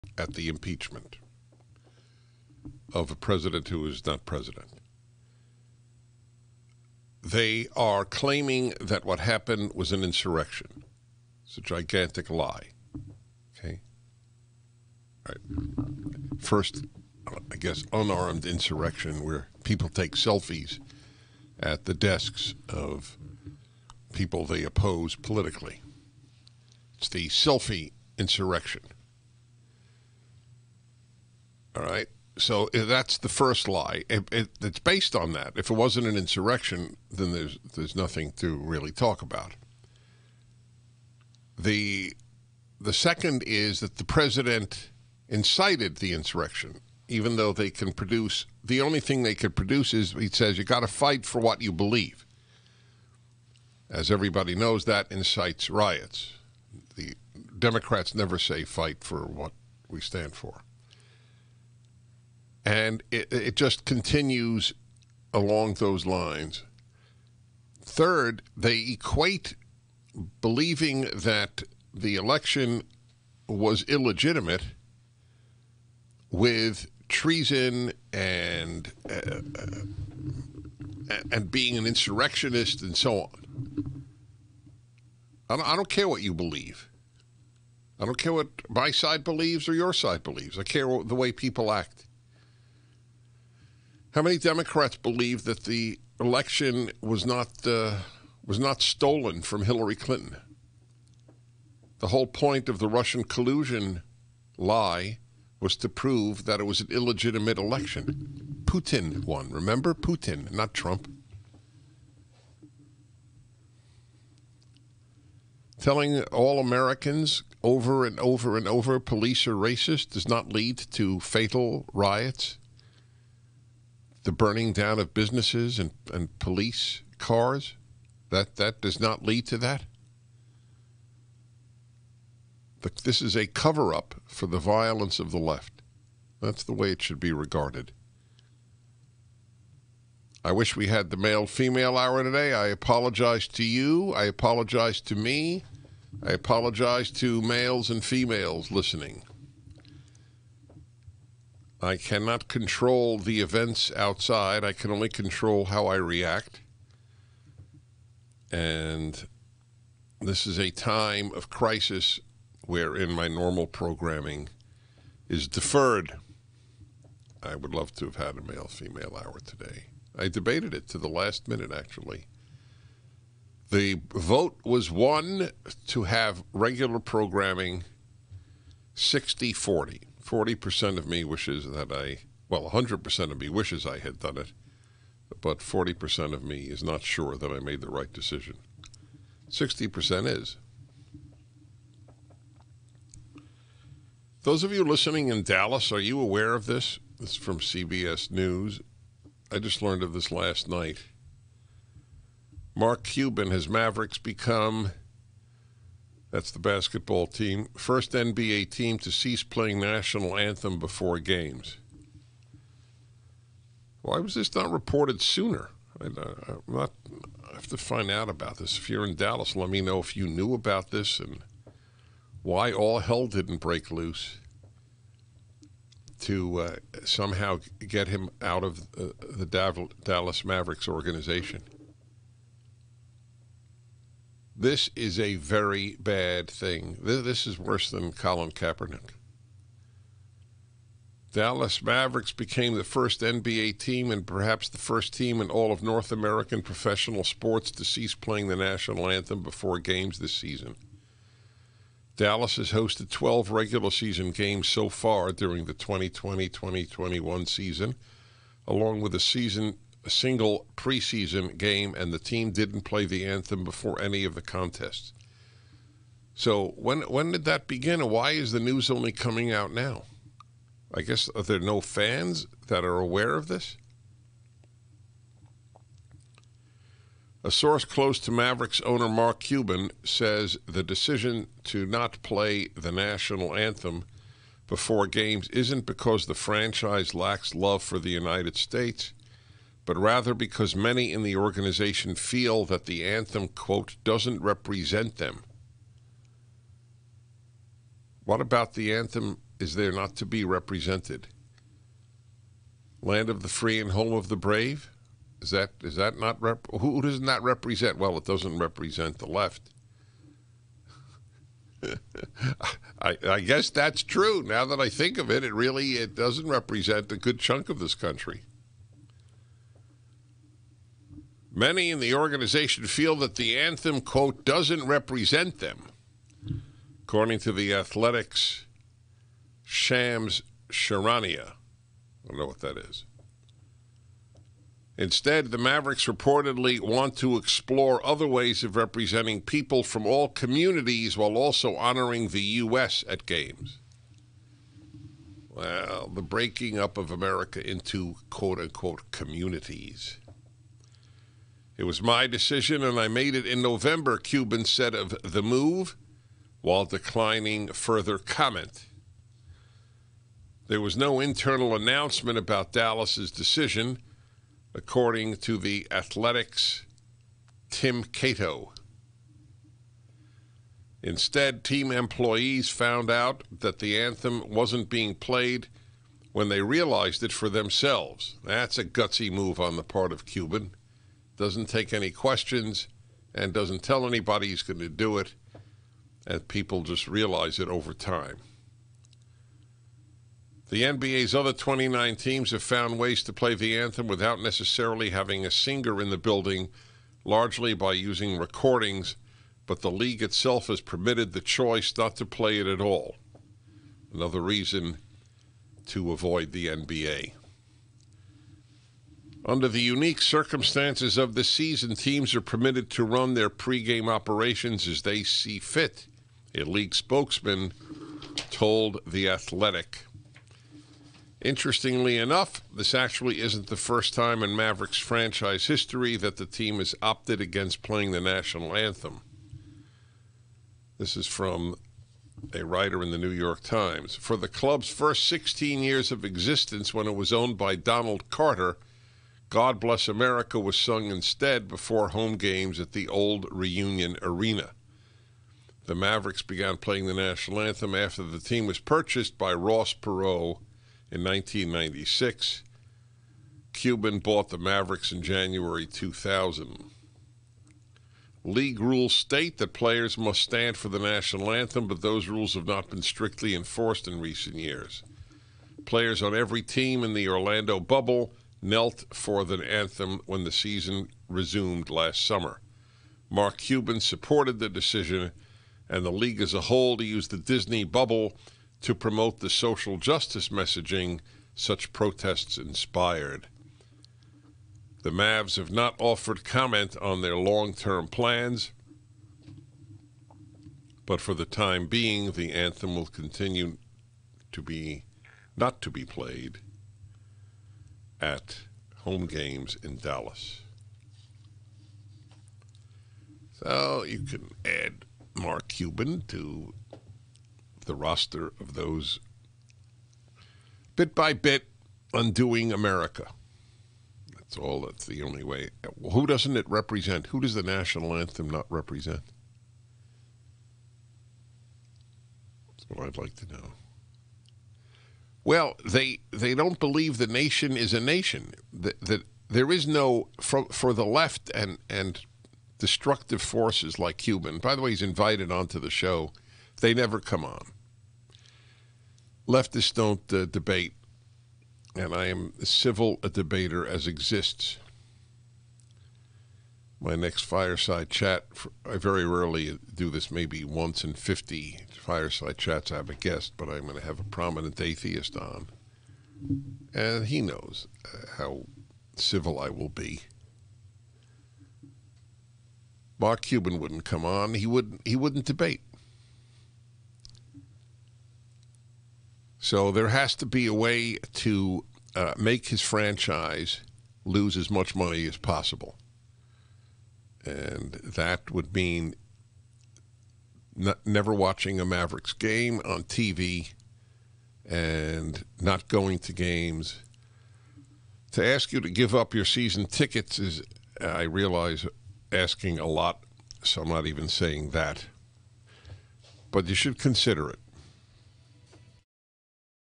The Lie About January 6th
This was not an insurrection.